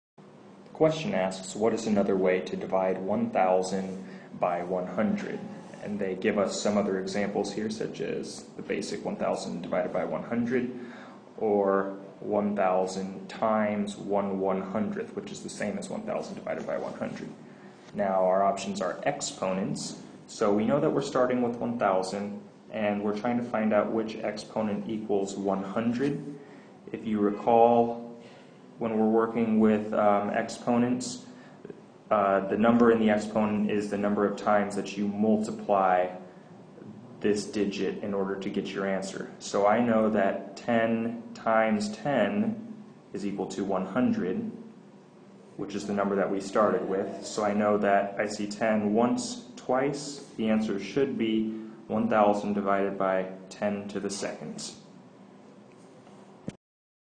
[5.NBT.3a-1.7] Decimals to thousandths - Common Core Standard
Dividing by 10^2 can be represented dividing by 100 which can be represented as a multiplication by 1/100